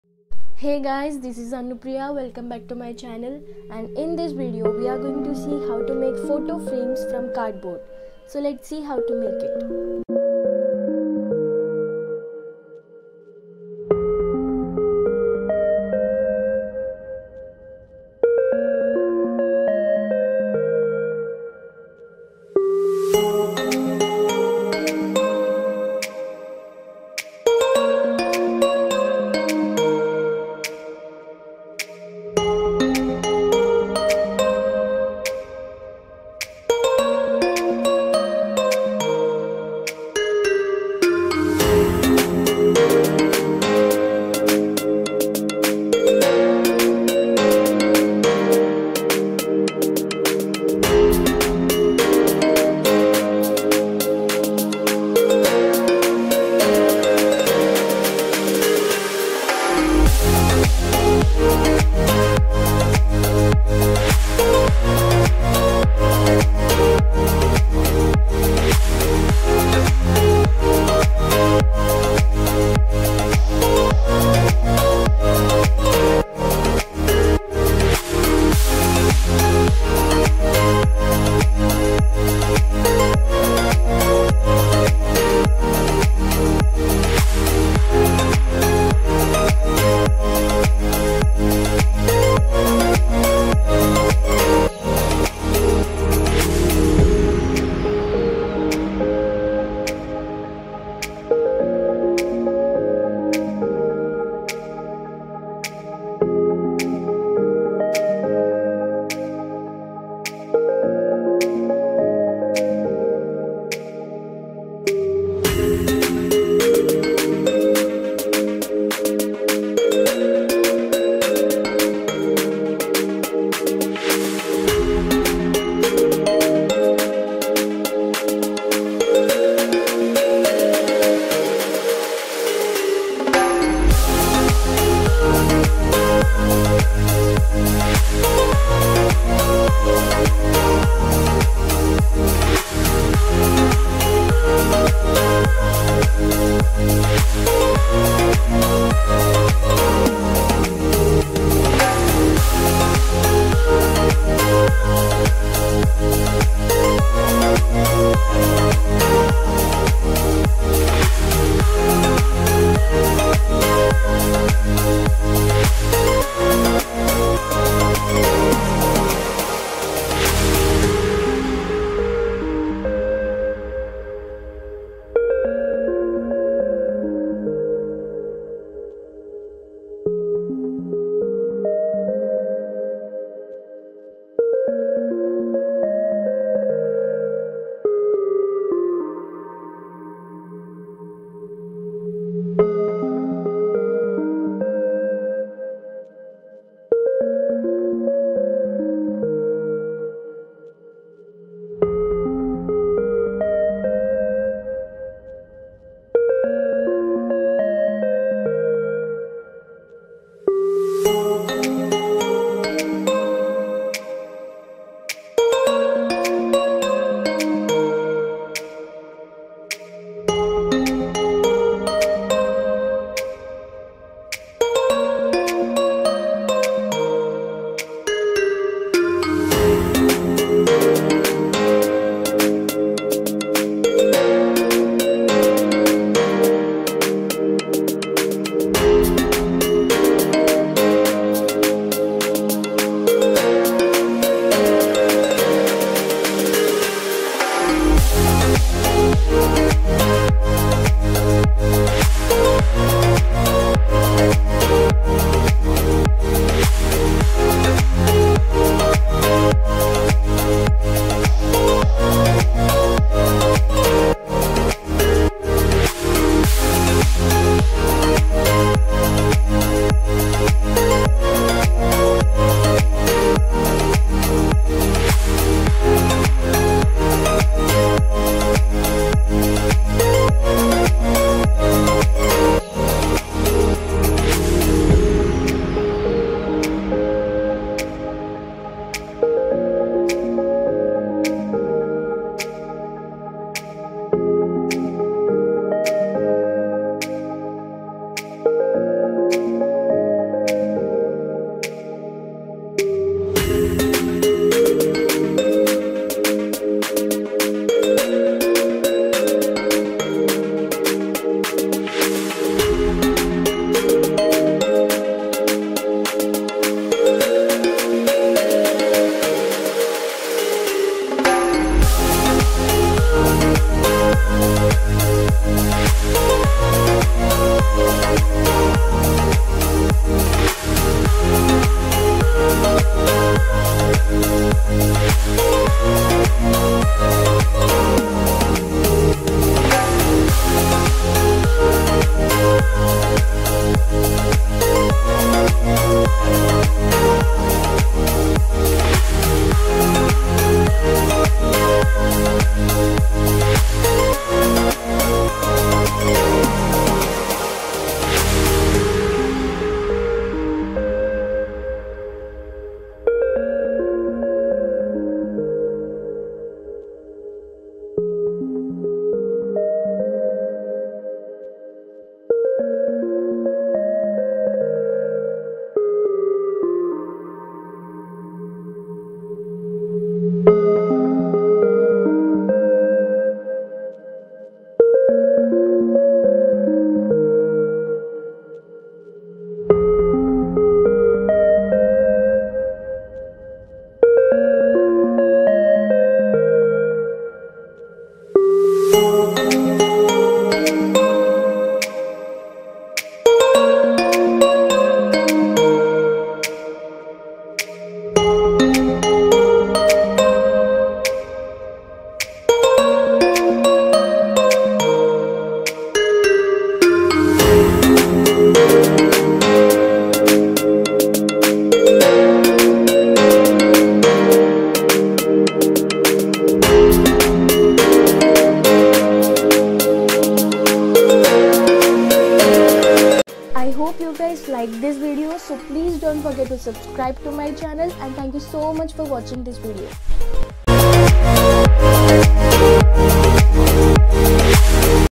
4 different photo frame DIY ideas || DIY photo frame with doodle painting
Hello guys!!

This video is about how to make photo frames with cardboard.
And painting in different ways... Like tape design and doodle painting
Requirements:
Cardboard, white paper, acrylic paints, sketch pens.
 you can also try this simple and amazing ideas at your home.